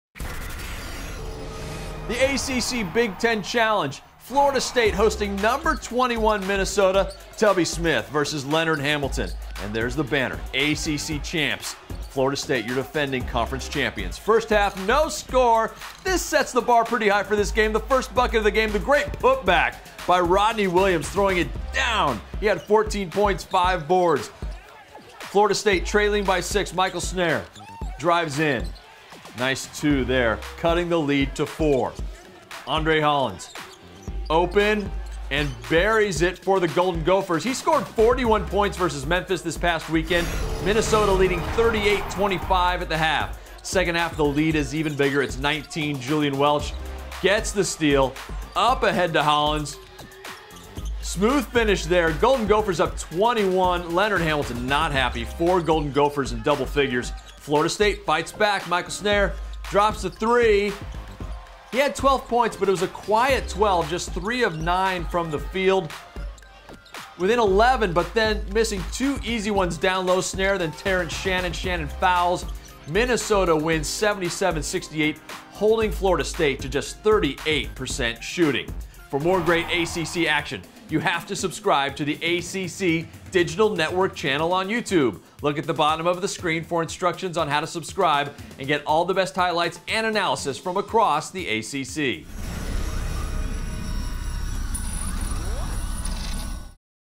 Minnesota vs Florida State Basketball Highlights - 2012
Florida State fell to 4-2 on the season with a 77-68 loss to Minnesota in the ACC-Big Ten Challenge. Terrance Shannon led the way for the Seminoles with 14 points and 11 rebounds.